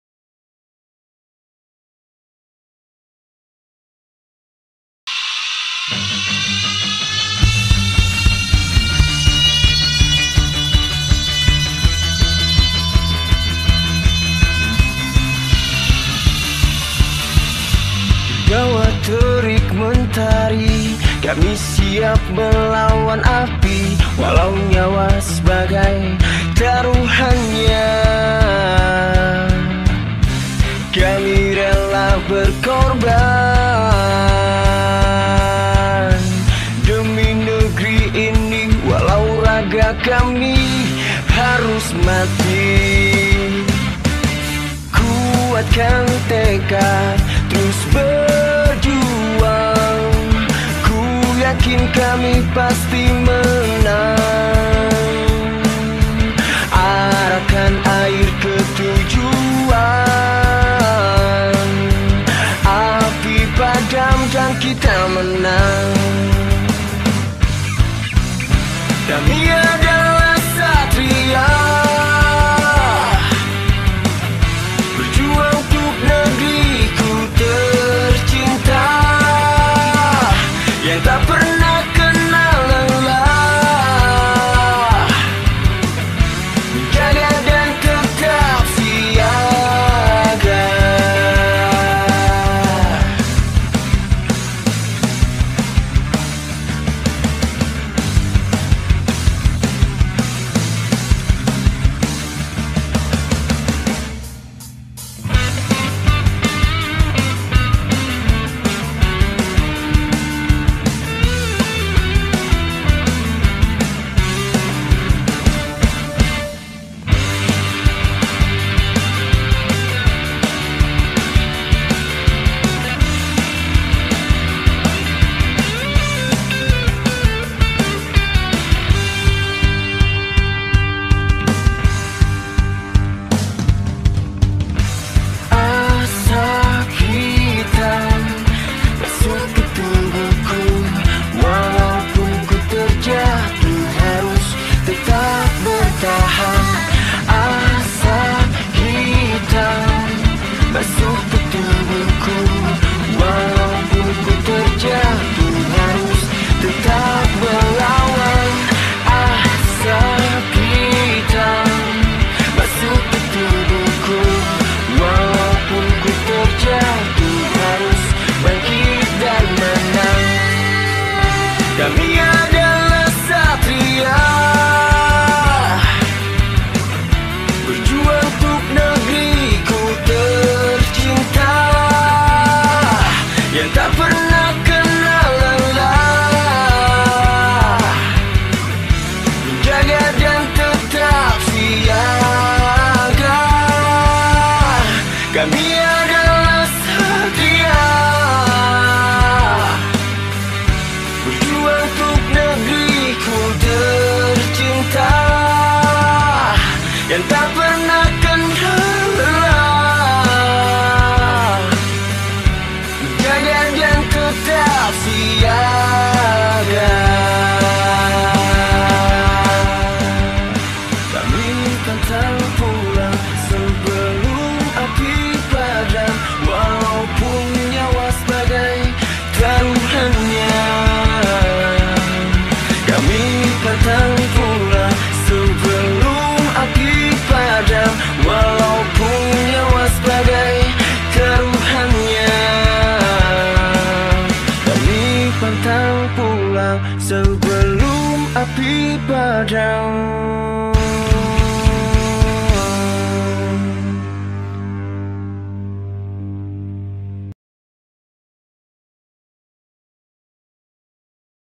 AKSI DAMKAR MANISE DALAM PADAMKAN API DI KEBAKARAN SOABALI AMBON
AKSI DAMKAR MANISE DALAM PADAMKAN API DI KEBAKARAN SOABALI AMBON pada tanggal 13 November 2021